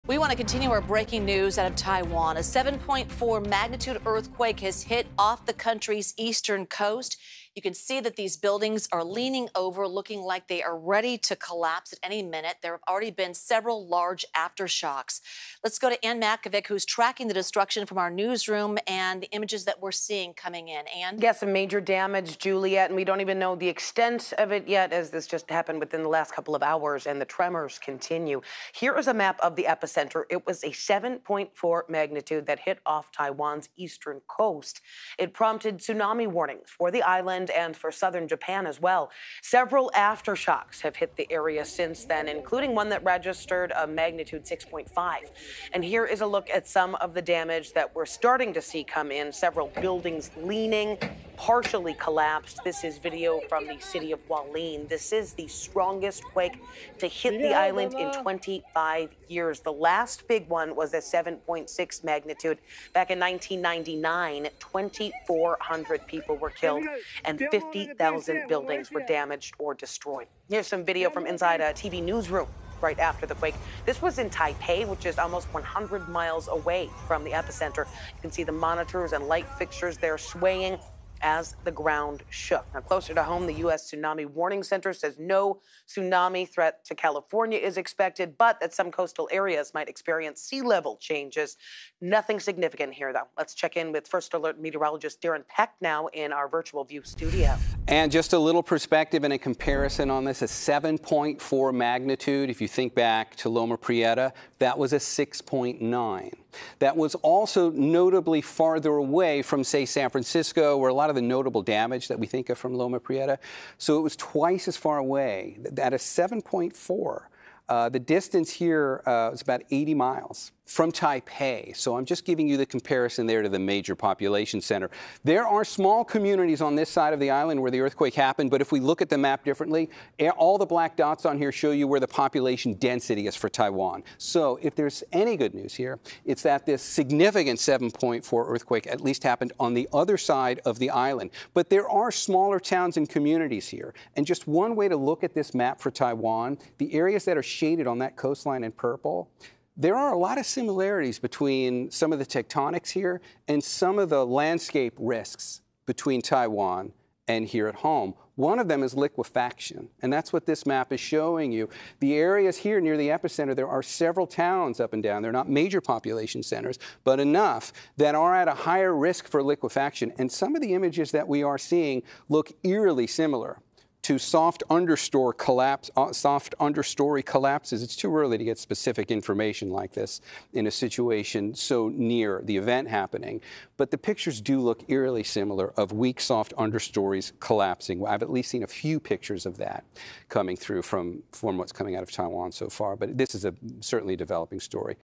7.4-magnitude earthquake hits near Taiwan, triggering tsunami warnings
Team coverage of impacts from magnitude 7.4 earthquake in Taiwan early Wednesday morning local time.